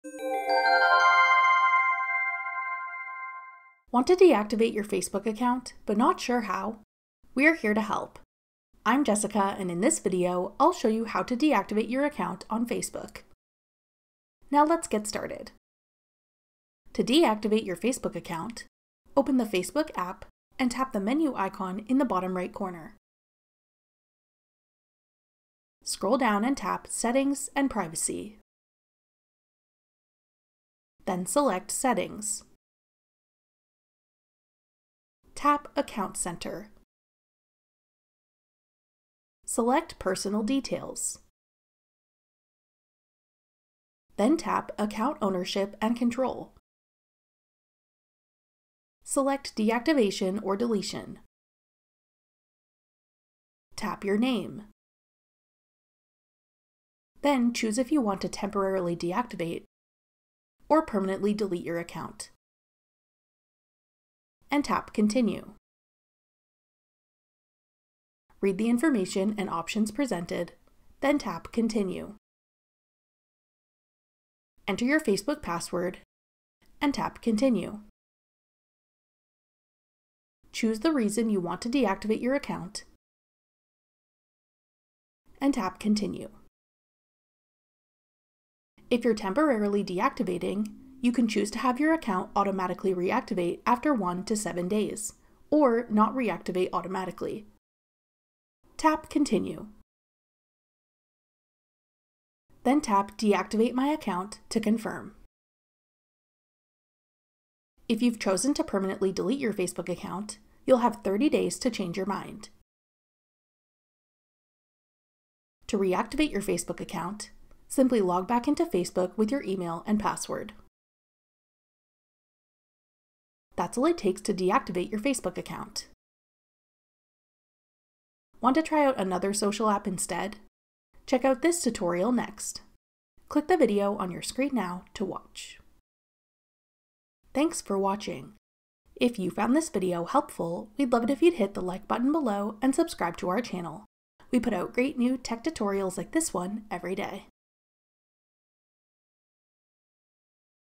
How to Deactivate Your Facebook Account in 2026 (Full Guide)
Want to deactivate your Facebook account but not sure how? We’re here to help. Watch this video to learn how to deactivate your account on Facebook.

To deactivate your Facebook account, open the Facebook app and tap the menu icon in the bottom right corner. Scroll down and tap Settings & Privacy. Then select Settings. Tap Accounts Center. Select Personal Details. Then tap Account Ownership and Control. Select Deactivation or Deletion. Tap your name. Then choose if you want to temporarily deactivate or permanently delete your account, and tap Continue. Read the information and options presented, then tap Continue. Enter your Facebook password, and tap Continue. Choose the reason you want to deactivate your account, and tap Continue. If you’re temporarily deactivating, you can choose to have your account automatically reactivate after 1-7 days, or not reactivate automatically. Tap Continue. Then tap Deactivate My Account to confirm. If you’ve chosen to permanently delete your Facebook account, you’ll have 30 days to change your mind. To reactivate your temporarily deactivated Facebook account, simply log back into Facebook with your email and password.